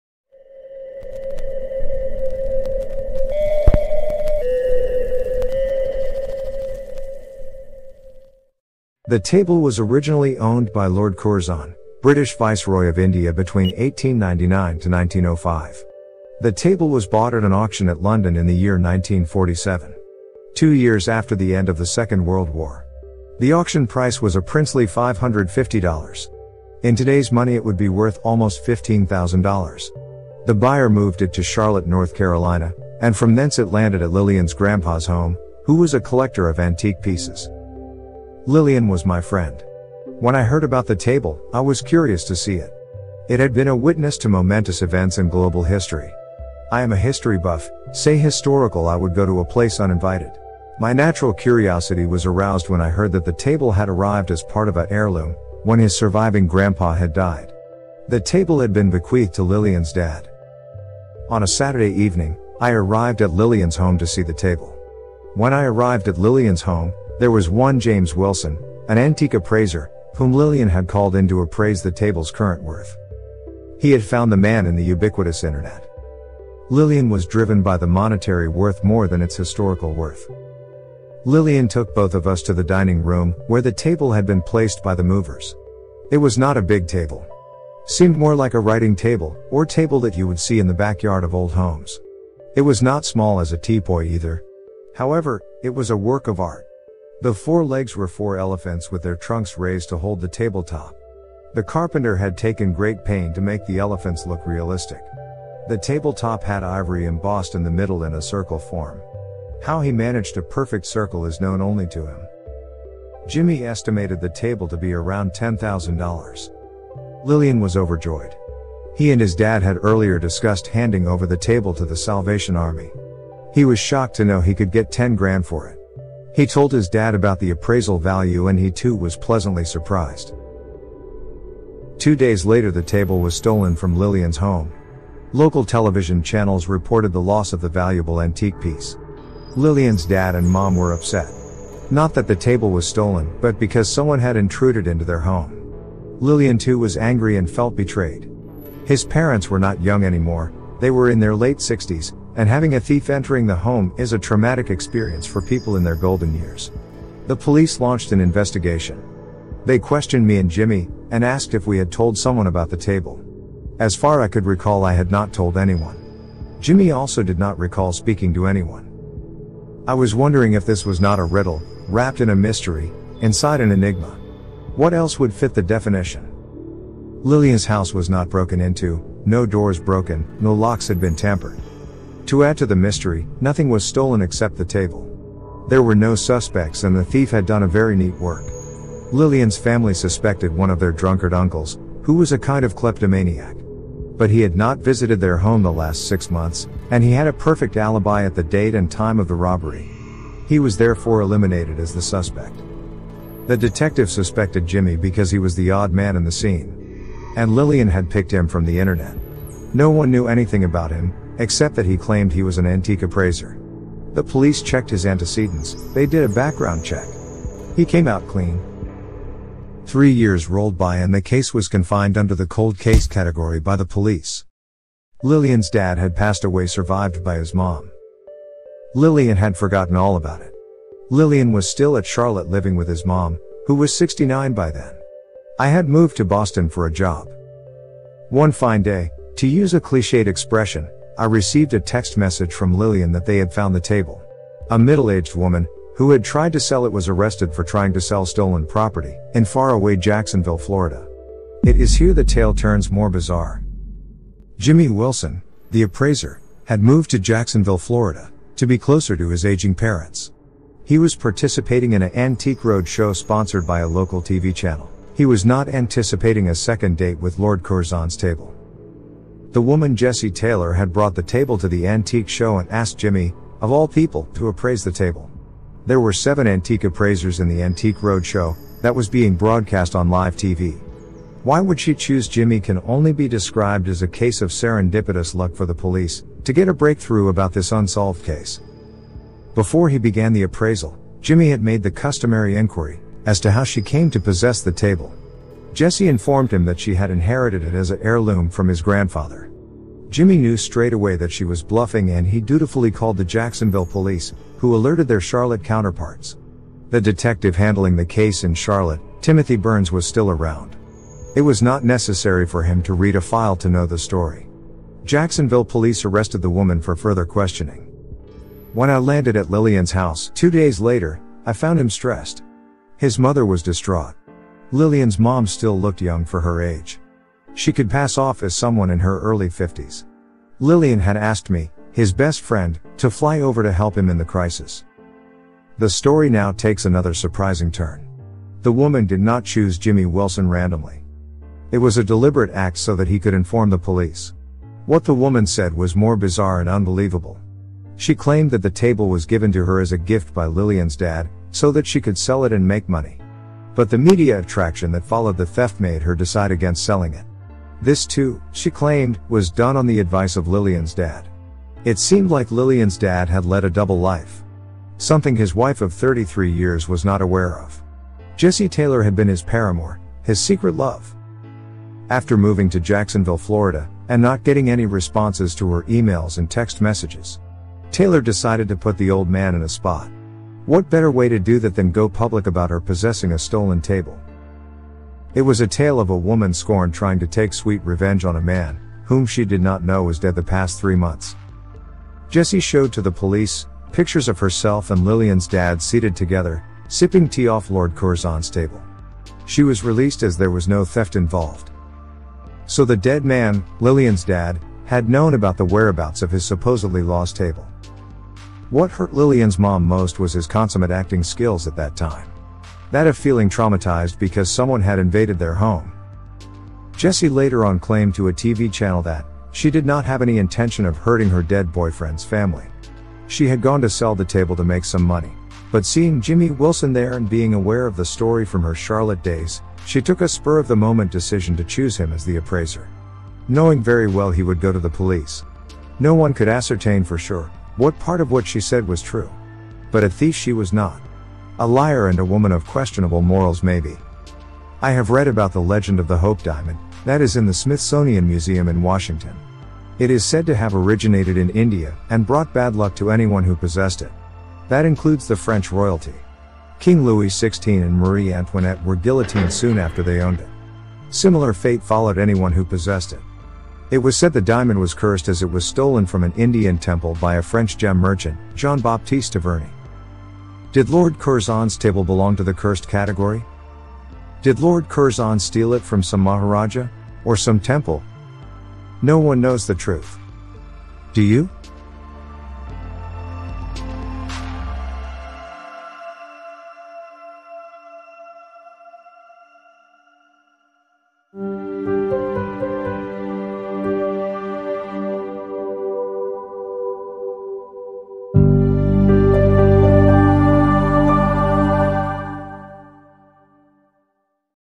LORD CURZON'S TABLE
Is Lord Curzon's Table cursed like the Hope Diamond?  Will it make the owner land in trouble?
A Story  By Award Winning Mystery Writer  Benjamin Thomas